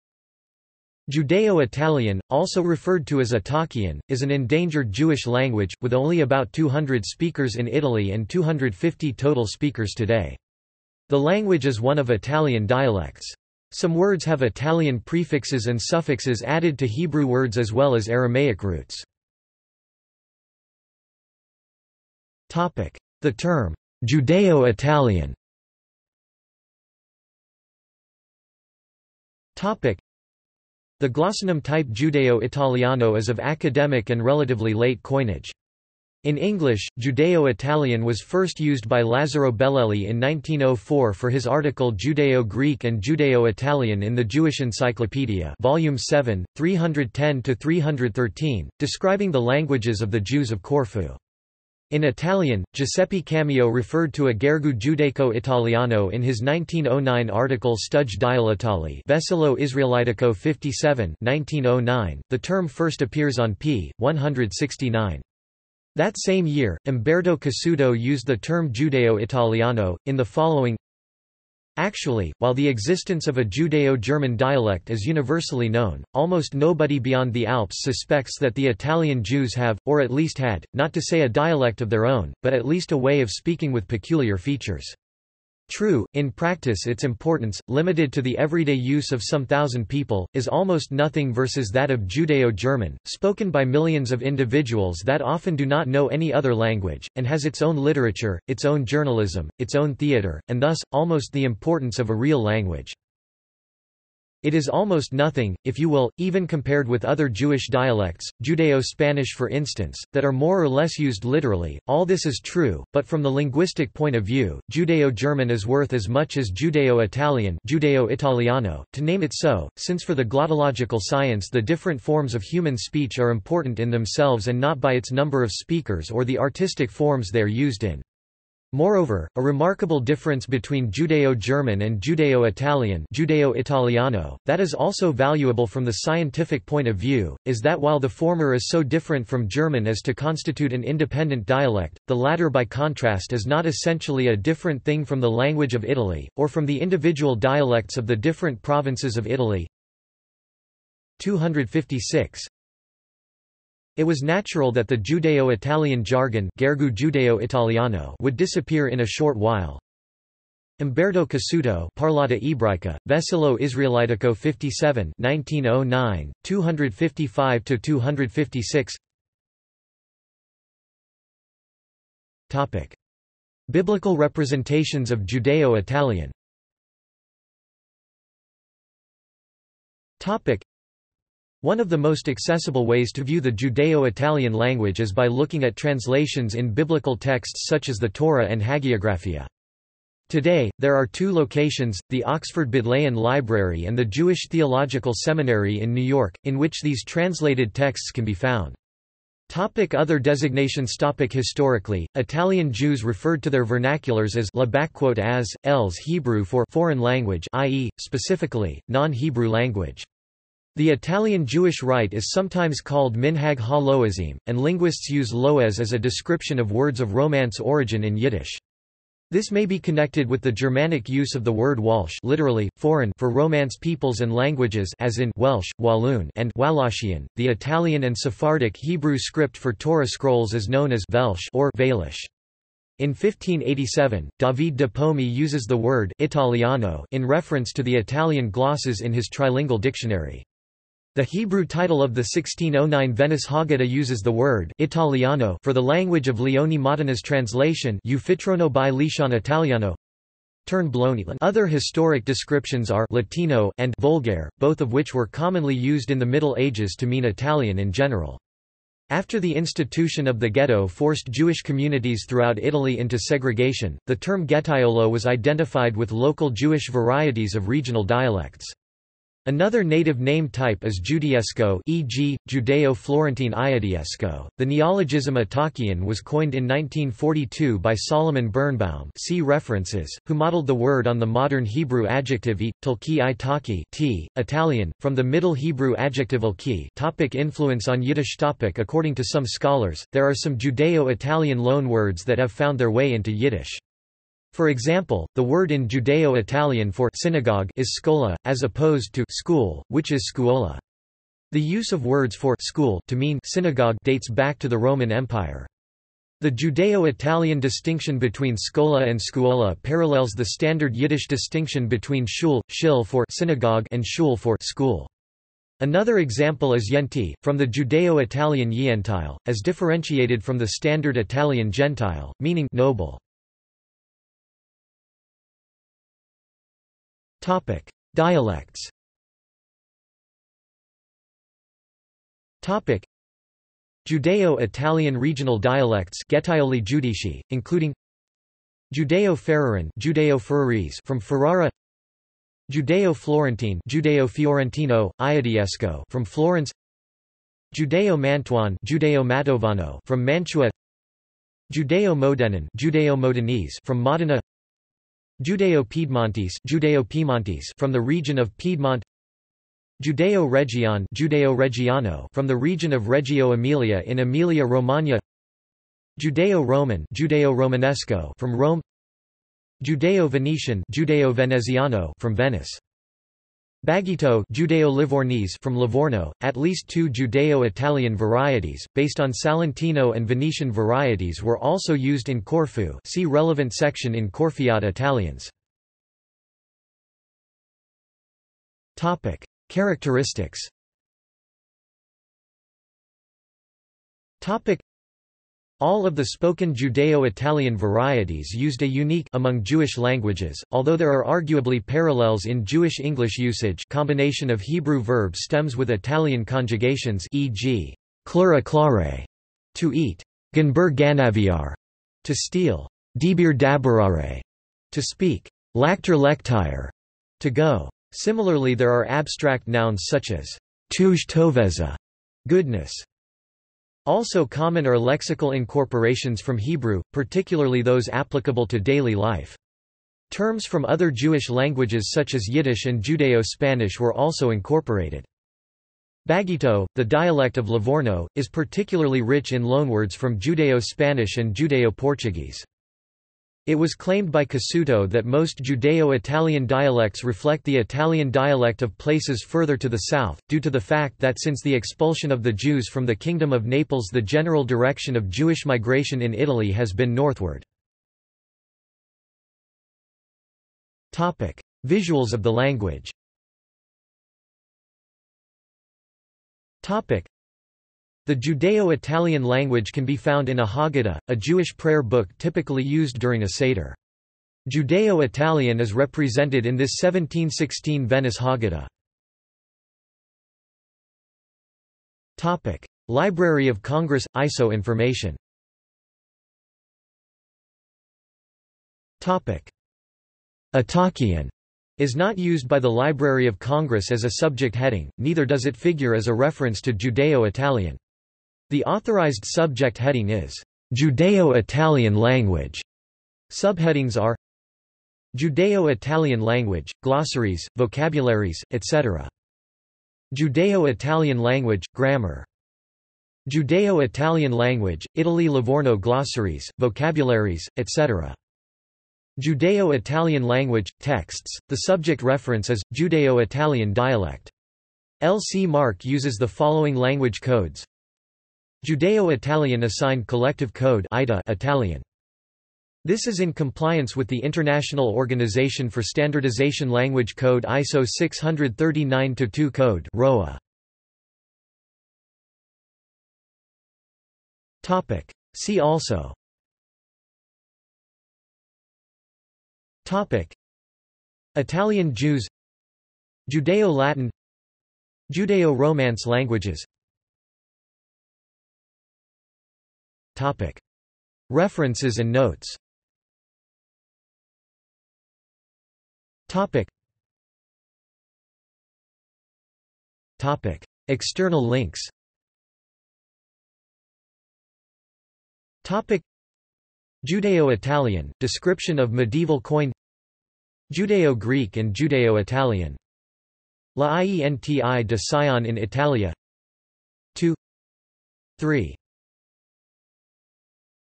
Italkian | Wikipedia audio article
This is an audio version of the Wikipedia Article:
Italkian

SUMMARY
Judeo-Italian, also referred to as Italkian, is an endangered Jewish language, with only about 200 speakers in Italy and 250 total speakers today. The language is one of Italian dialects. Some words have Italian prefixes and suffixes added to Hebrew words as well as Aramaic roots.